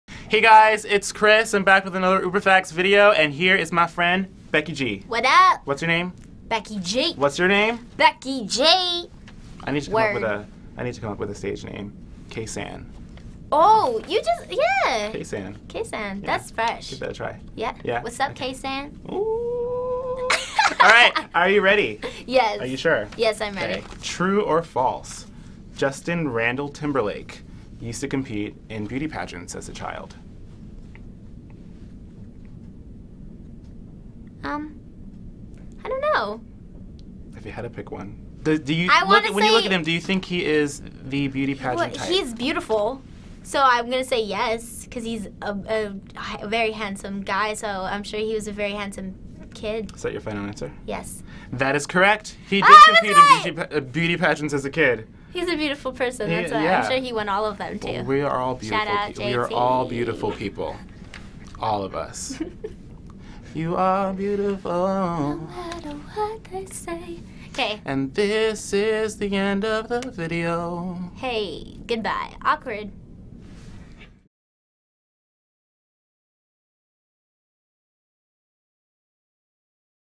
Does Becky G. Think Justin Timberlake Was In Beauty Pageants?
Becky G. stops by for an Uber Music Facts trivia session at 102.7 KIIS-FM in Los Angeles.